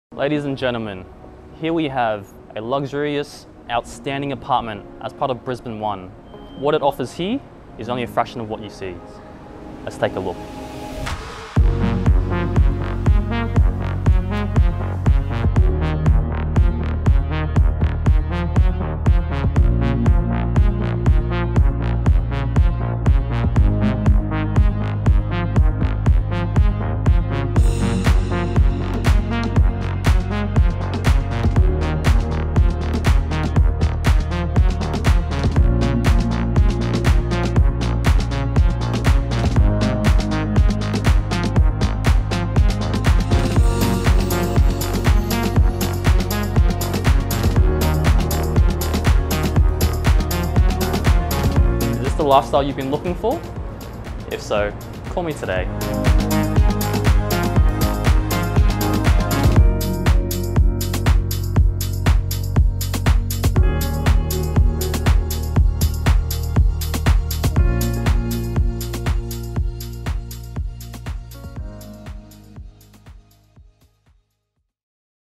32803/1 Cordelia St, South Brisbane
Just wait until you see the stunning views from this brand new, luxury apartment in central South Brisbane - it's truly a gorgeous spot to call home and you can be its first resident.